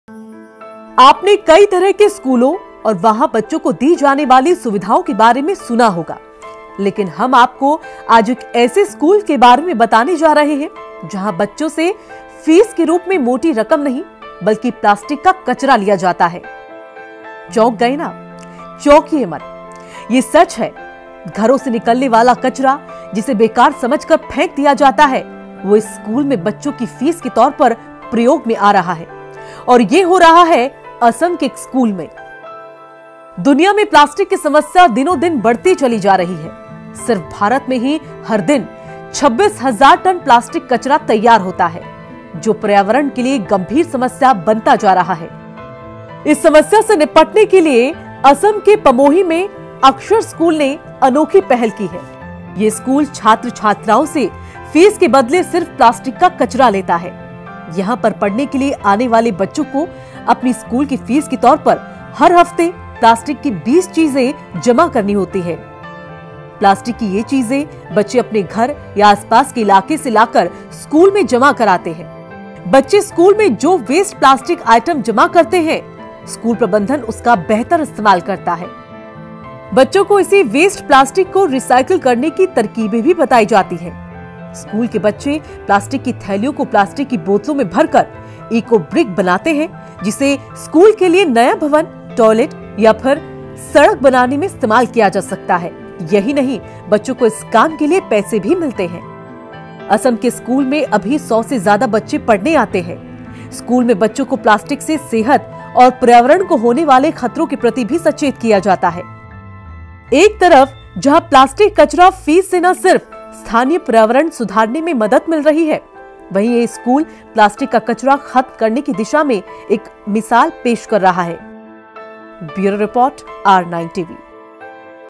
ये कचरा बड़े काम का है..
असम के पमोही का ‘अक्षर’ स्कूल जहां स्कूल फीस के बदले बच्चों से लिया जाता है प्लास्टिक कचरा…और बच्चों को सिखाया जाता है पर्यावरण विज्ञान.